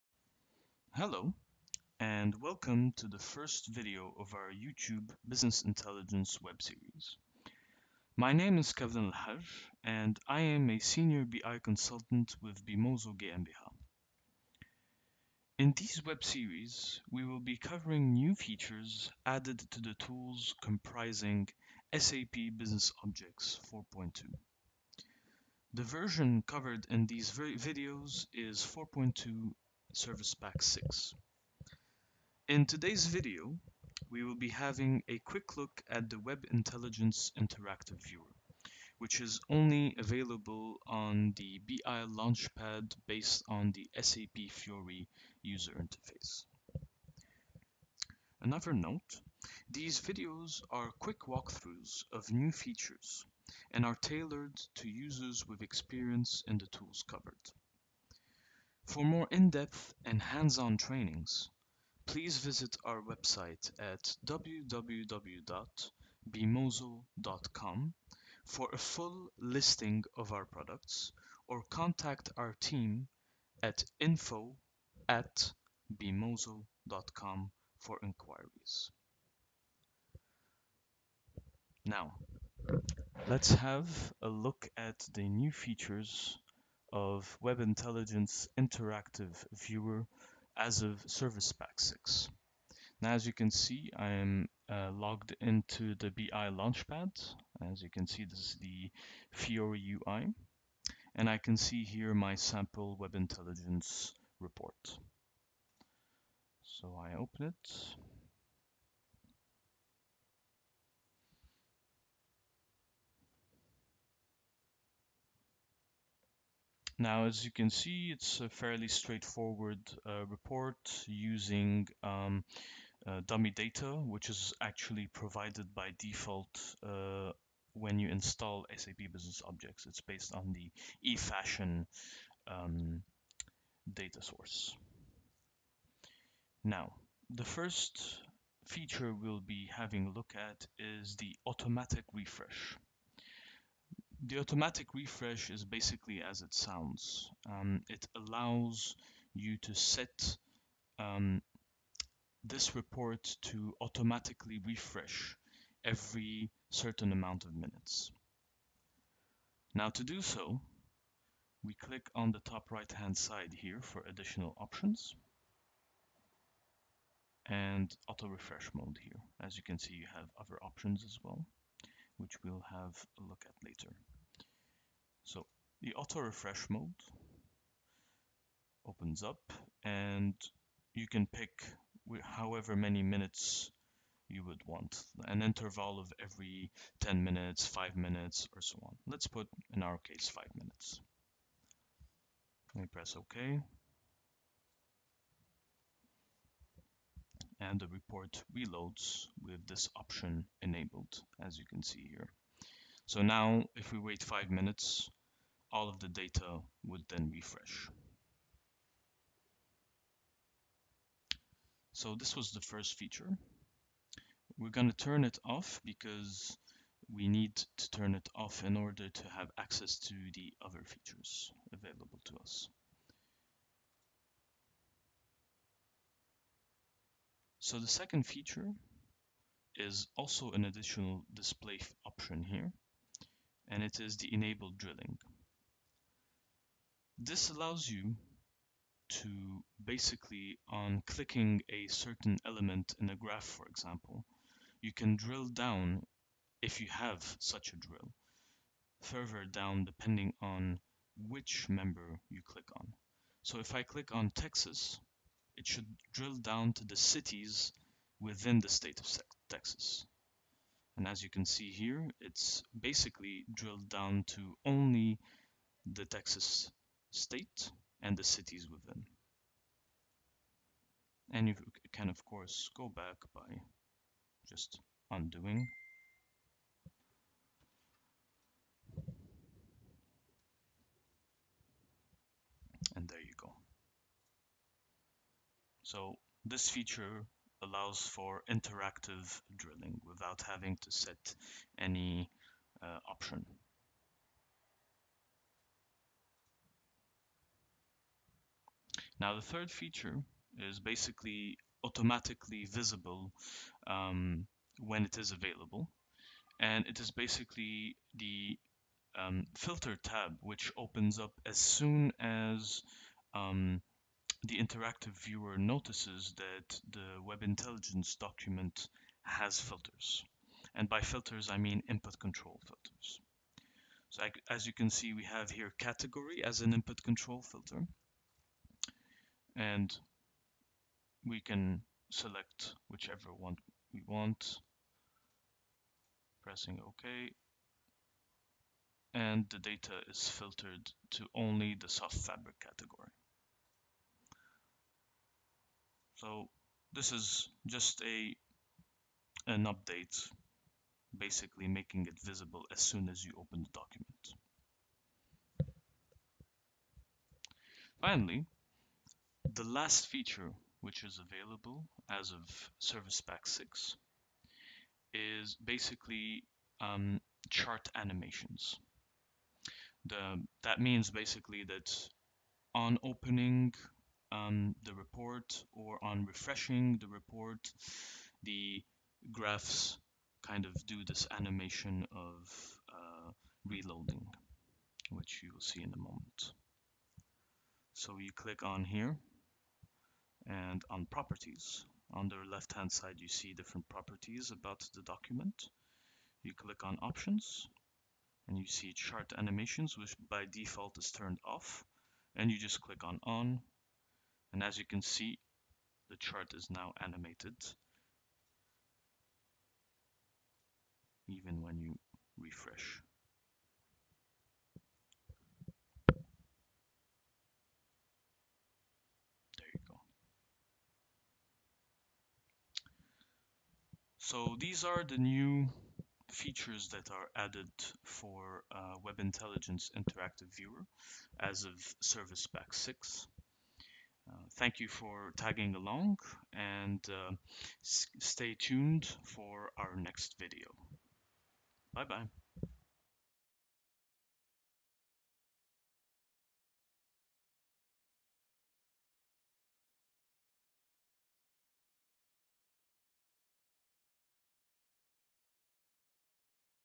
SAP Web Intelligence 4.2 SP6 business objects New:1.Interactive Viewer
In this Web Series we explore the new features of Version SAP BusinessObjects Web Intelligence 4.2 SP6.

This Video introduces new components added to the Web Intelligence Interactive Viewer. The Interactive Viewer is an option that allows users to view their Web Intelligence Documents in a more dynamic, modern and visually appealing way.